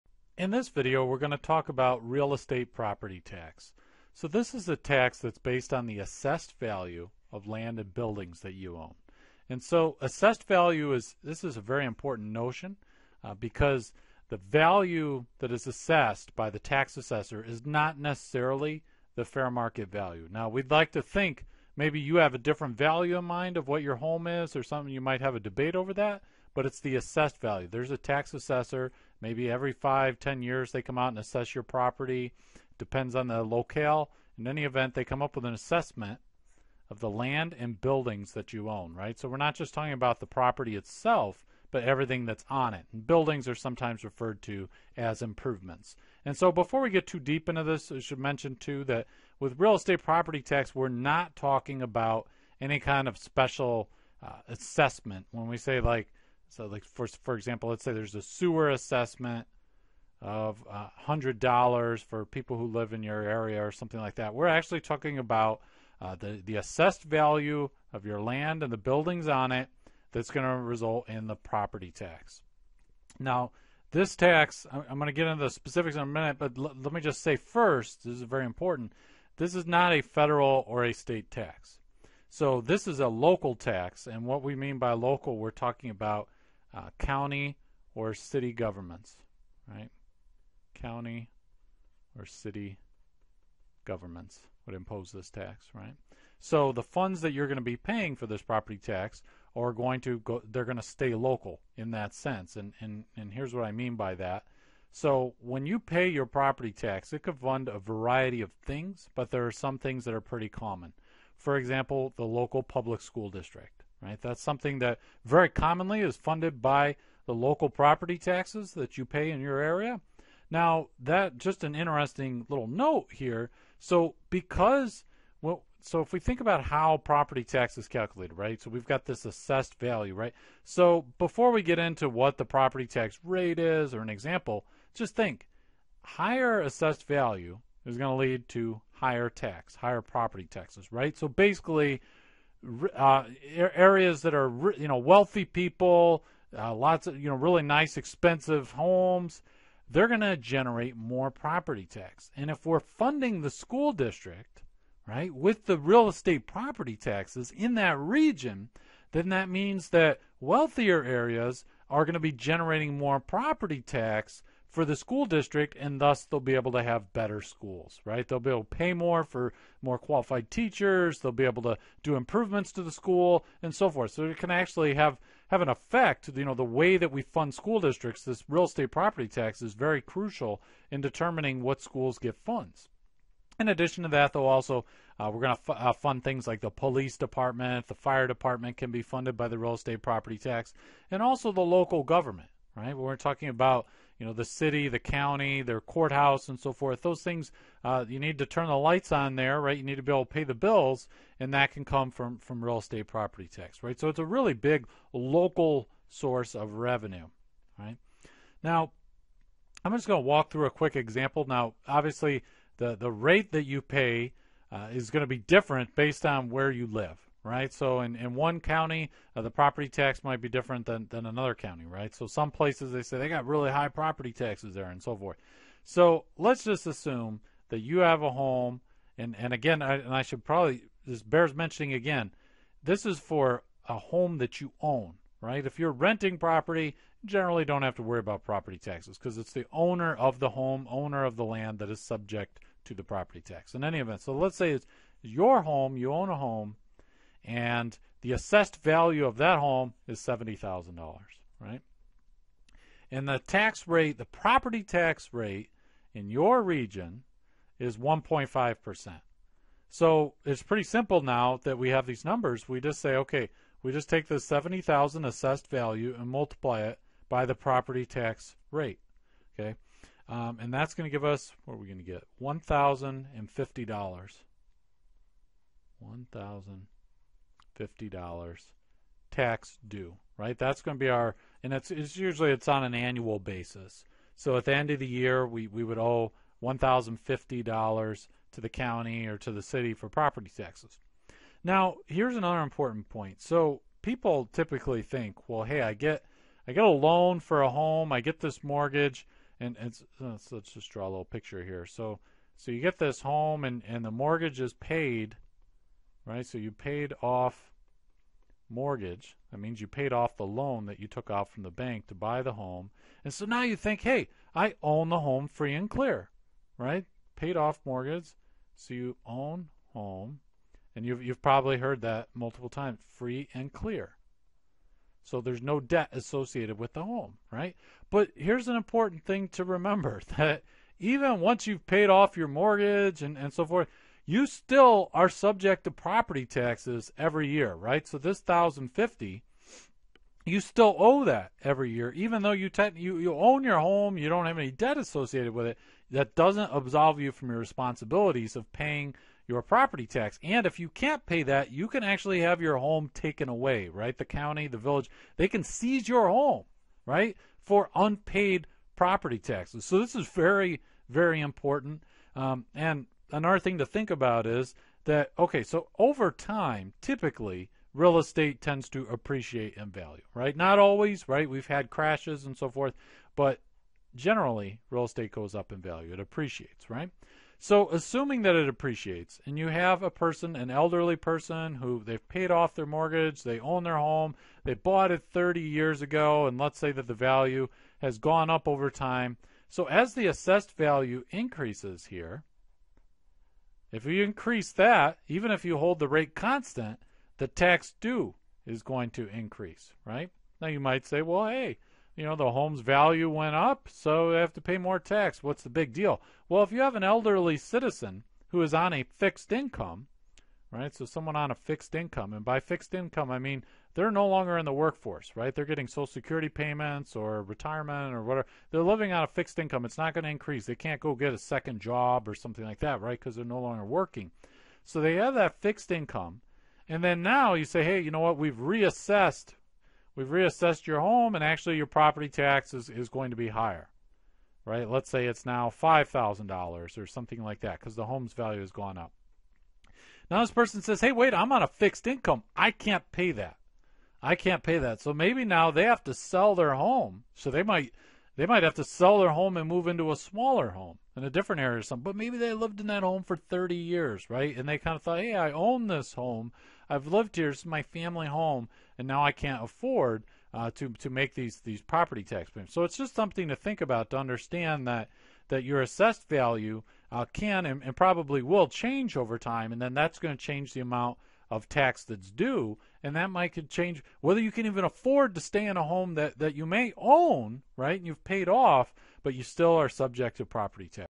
Real Estate Property Tax
This video discusses the concept of real estate property tax.  The video explains what real estate property taxes are and demonstrates how to calculate real estate property tax with an example.  The video also discusses how real estate property taxes are an important source of revenue for public school districts, police and fire protection, and local governments.  In addition, the video discusses how property tax re-assessments can lead to issues for people living on a fixed income, and how local governments have the power to seize a person's home as a result of unpaid real estate property tax.—
ACCESS INDEX OF VIDEOS
CONNECT WITH EDSPIRA
CONNECT WITH MICHAEL
ABOUT EDSPIRA AND ITS CREATOR